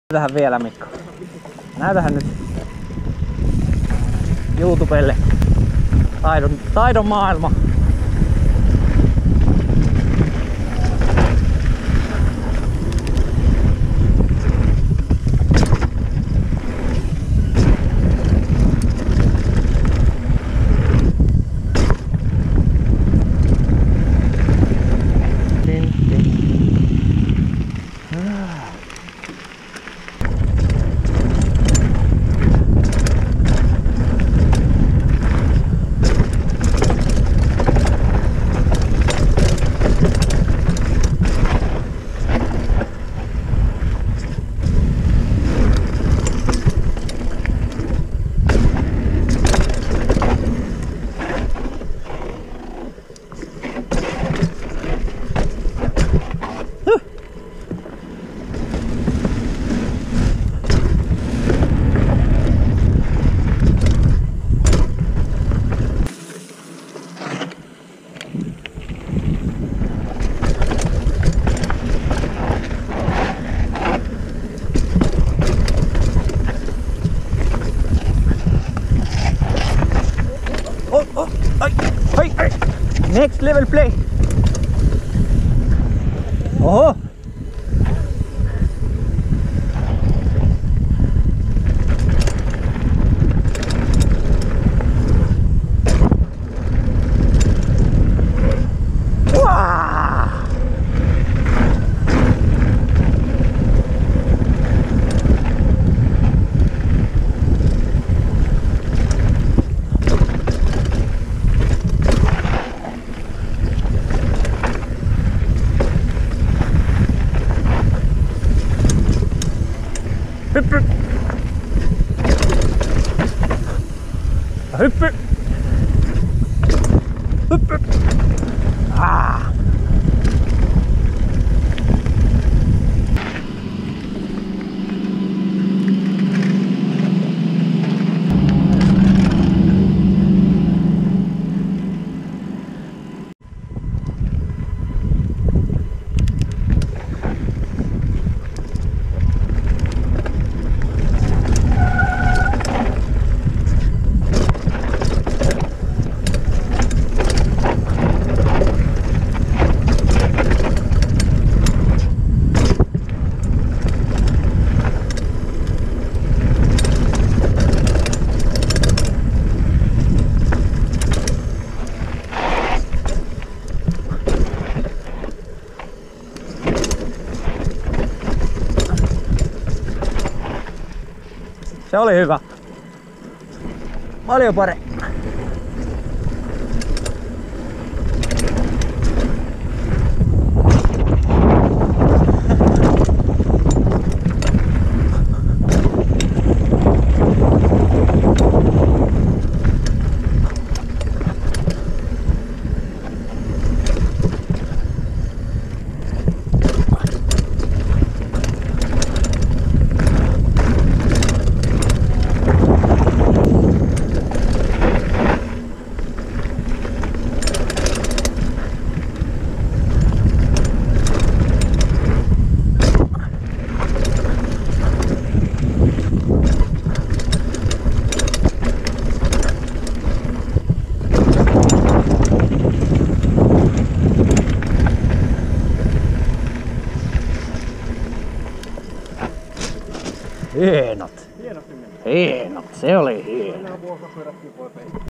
First timers at Sappee Bikepark doing Downhill
Bikeparks and downhill might feel quite scary when you look at people's videos so I decided to upload one as I am at a very beginner level phase. As you can see even a beginner can have fun doing so. Me and my buddy Mikko went to test it out and had quote a fun time.

Sappee Bikepark in Finland is quite entry-level or at least you can find easy routes if you are scared of the harder routes and dont want to get yourself hurt.

I was running on Focus Bold Factory 27.5+ which is a hardtail. Mikko had a Full Suspension one.